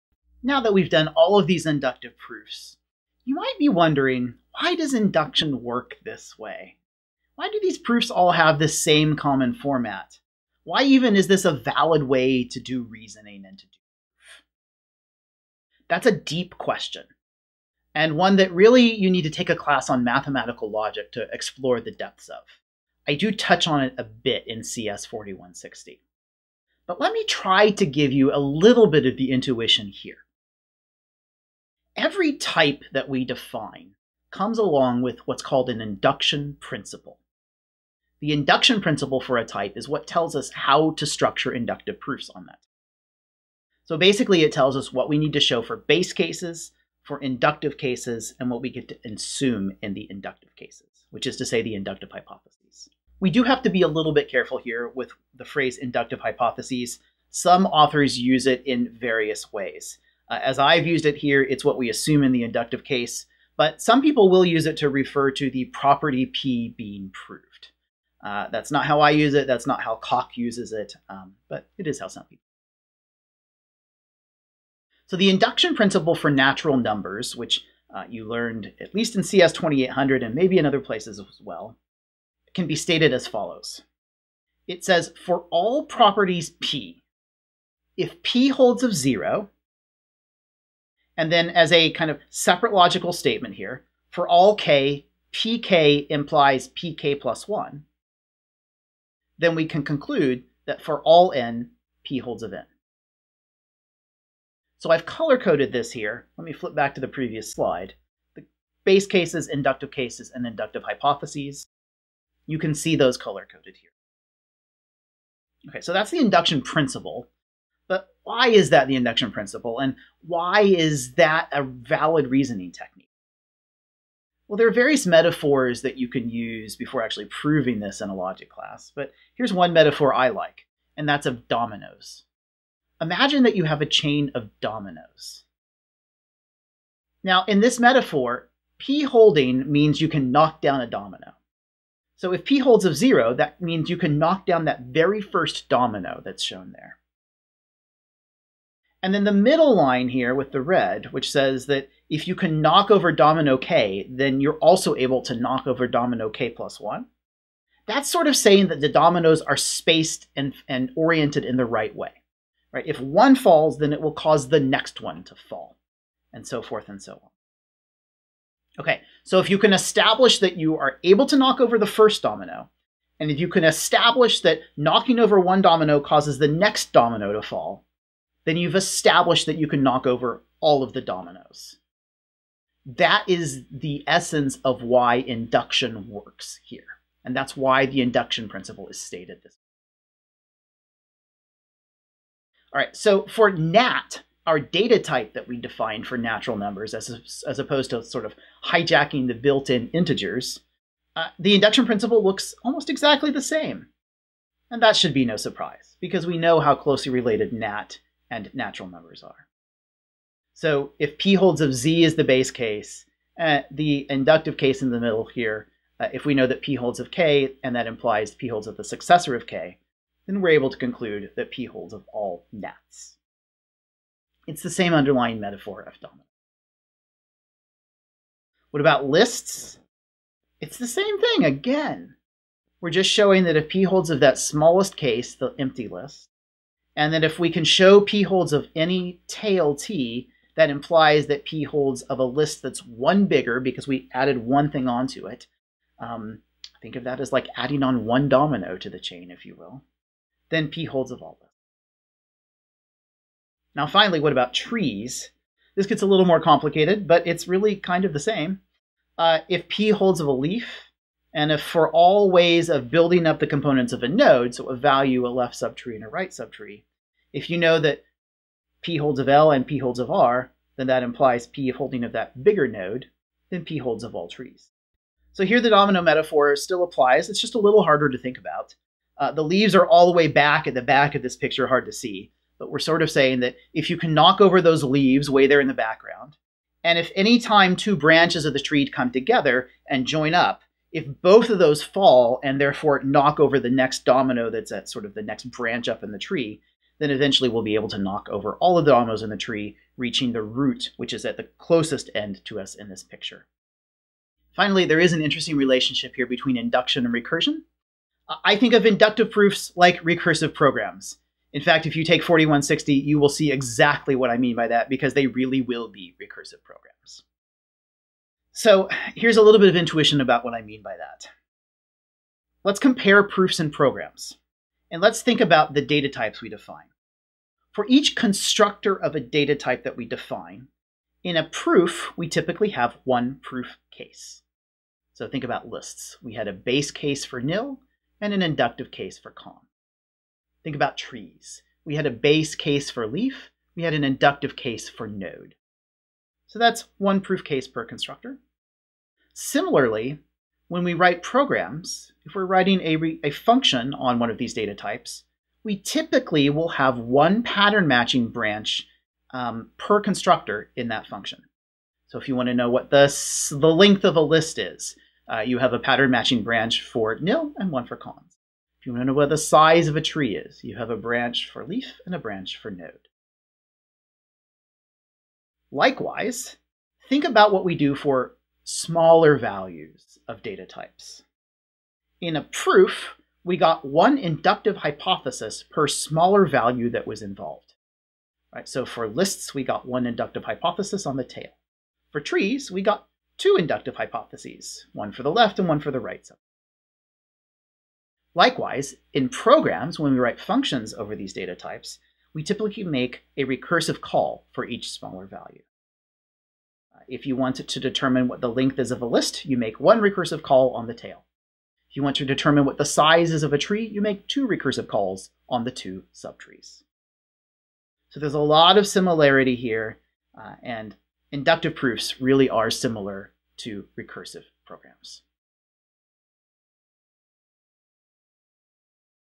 Induction and Recursion | OCaml Programming | Chapter 6 Video 32
A comparison of inductive proofs and recursive programs